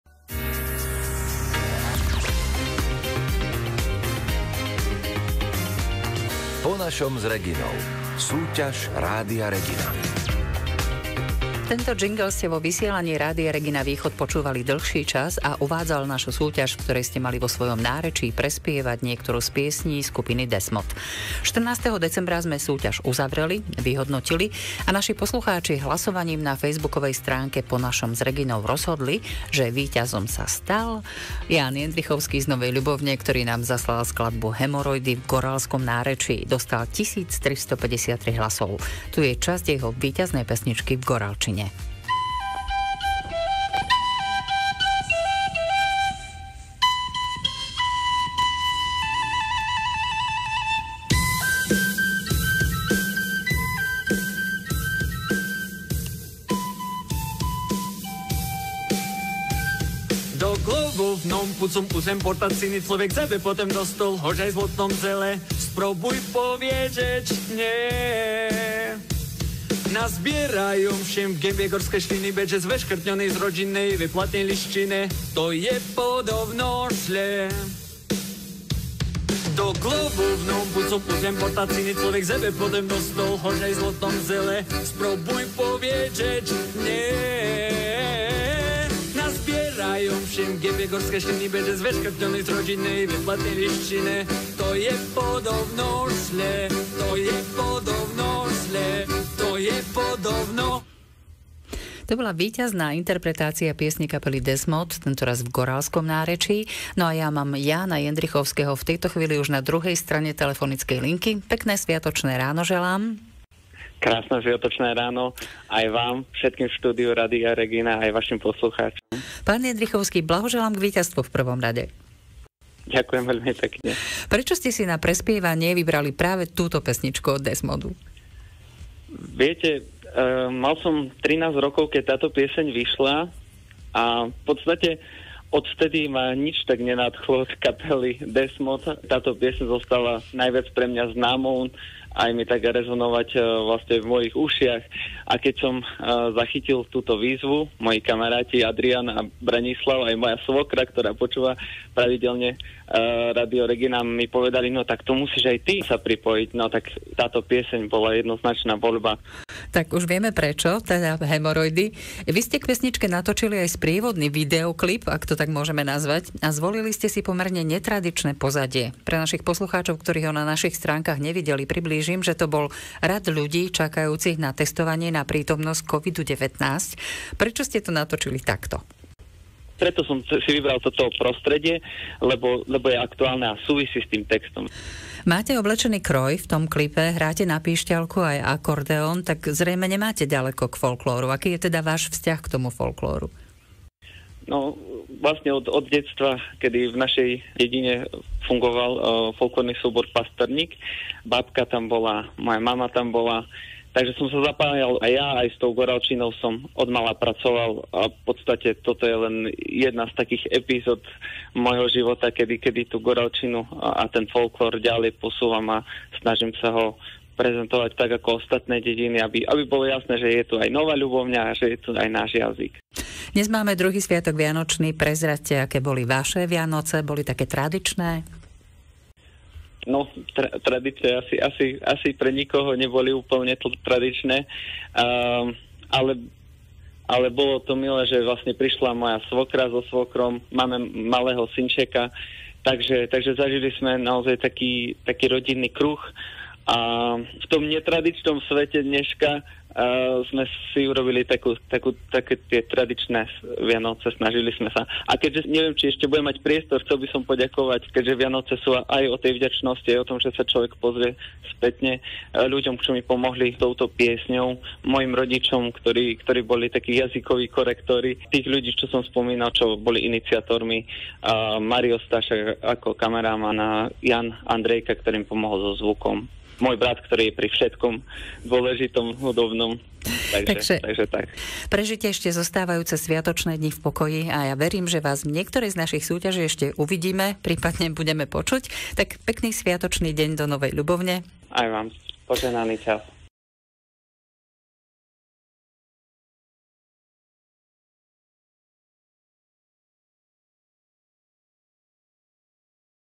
Novoľubovnianska goraľčina na rádiu Regina opäť
Zostrih s rozhovoru Jána Jendrichovského v relácii rádia Regina.